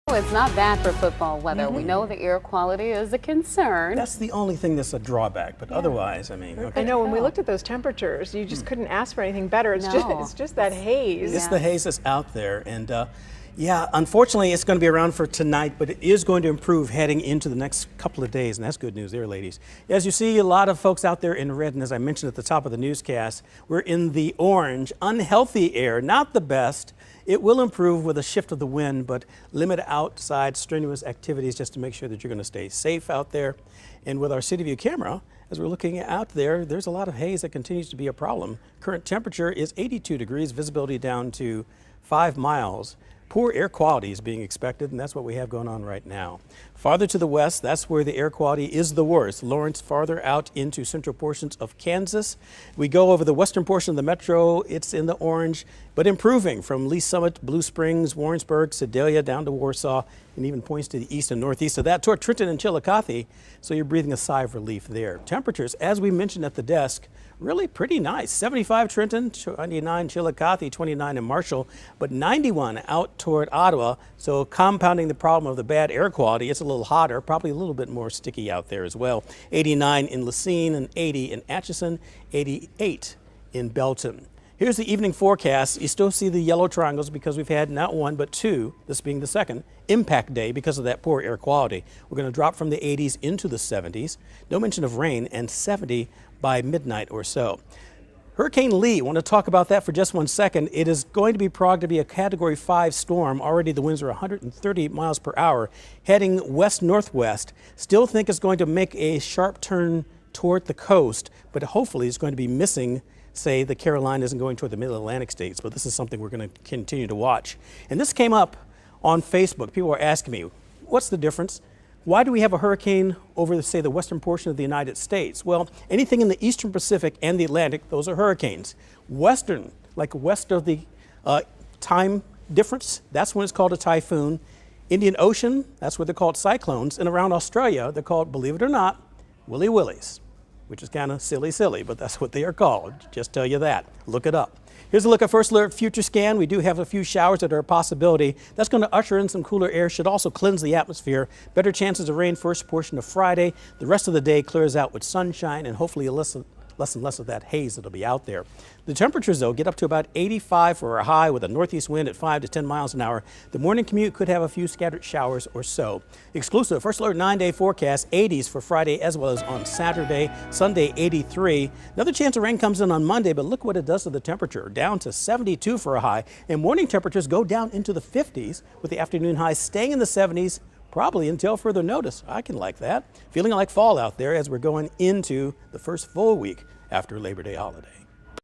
Poor air quality ruins otherwise great football weather
Poor air quality ruins otherwise perfect football weather